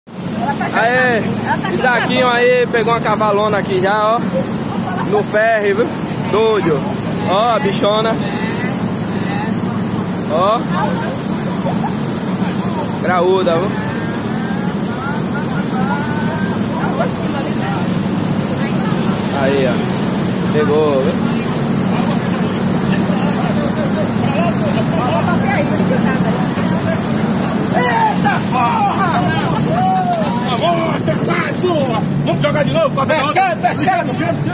Pescaria ferry boat Salvador dia 11/04/2019 cavala de 6kg
Ferry de Salvador Bahia cavala 6 kg